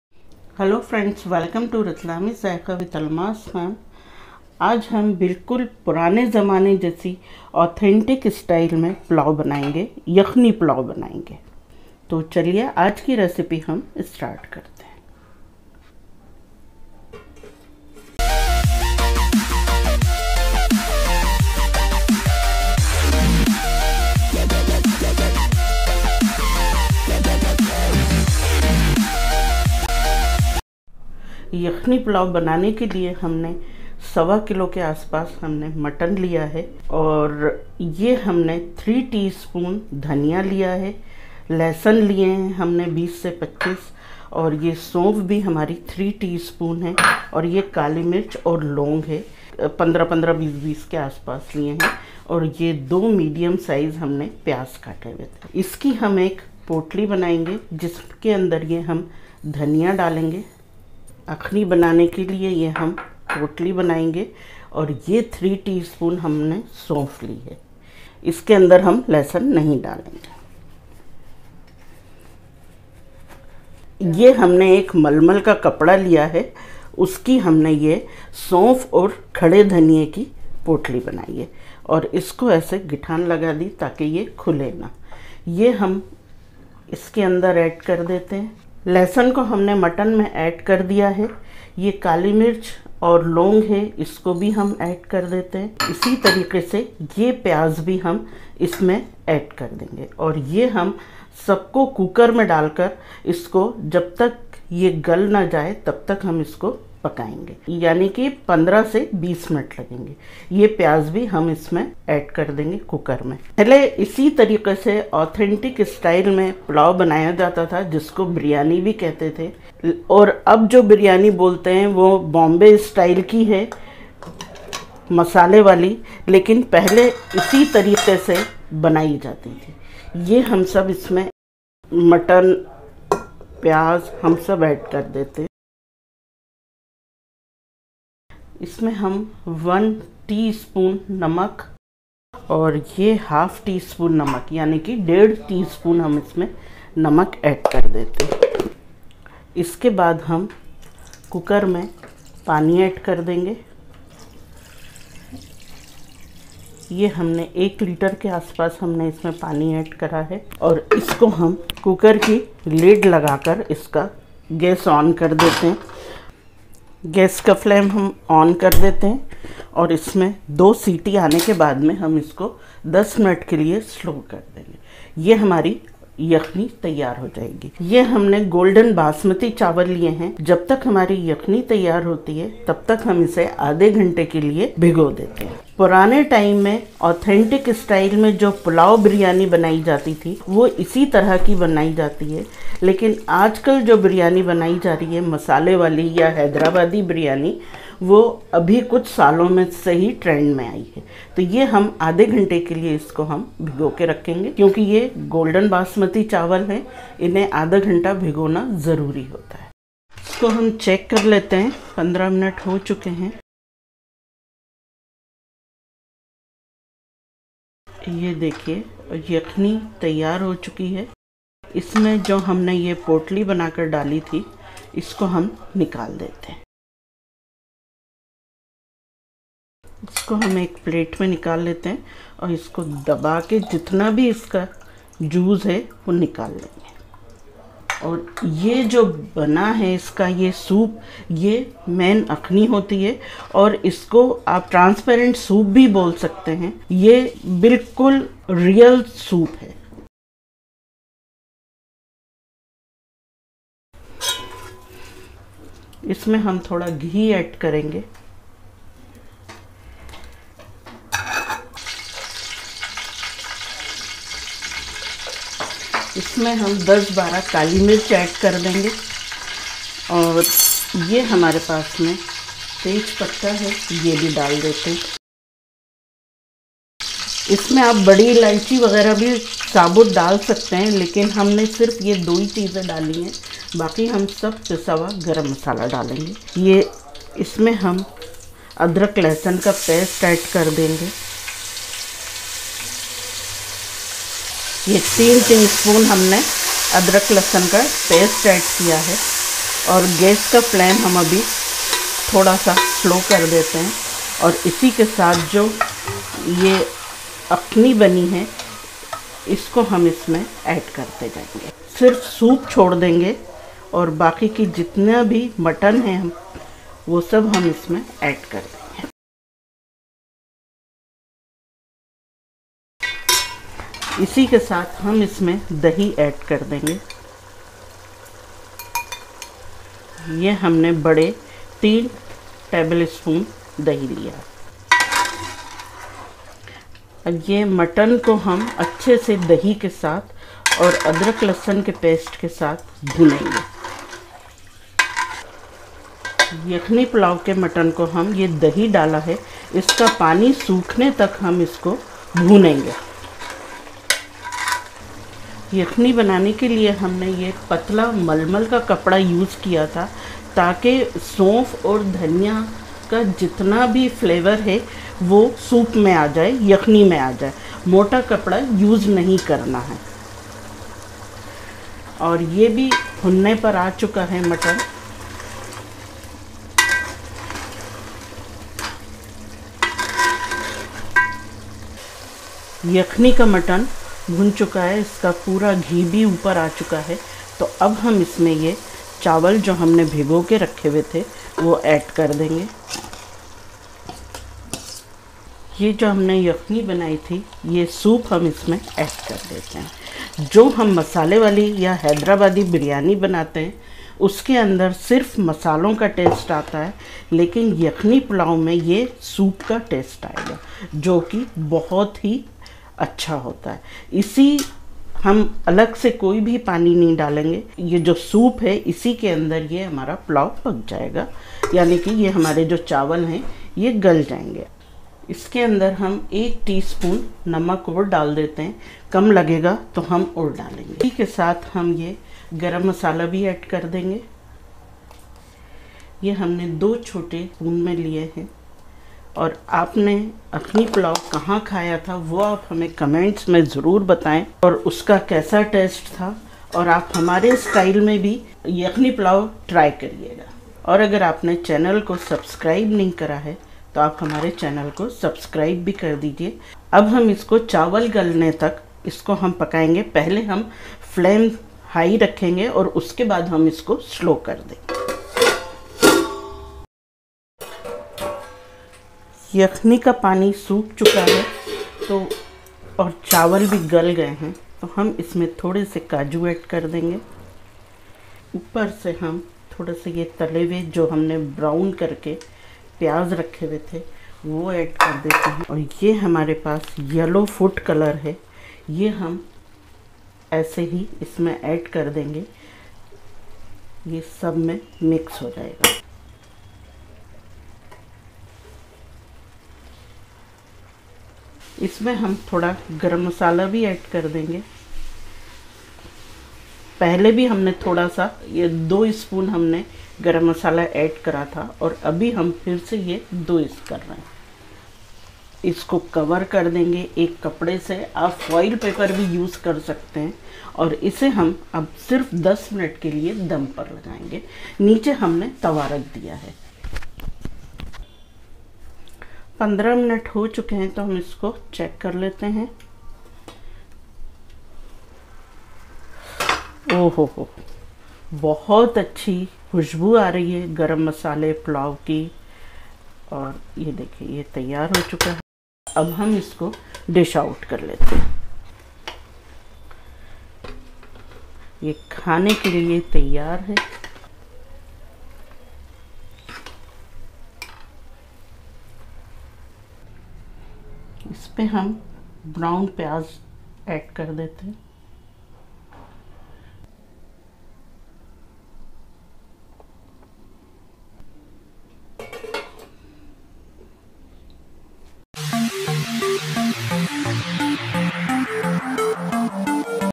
Mutton Yakhni Pulao eid or dawat wala | मटन पुलाव रेसिपी यखनी के साथ | By Almas Khan
Hello & Welcome to Ratlami Zayqa !!
I am Almas Khan and this is my food channel.

Learn How to make Authentic style Mutton Pulao Recipe, Its very juicy and delicious Must Try this mutton pulao at your Home. A good Yakhni is important for a good Yakhni Pulao. Here's my recipe for Mutton Yakhni Pulao.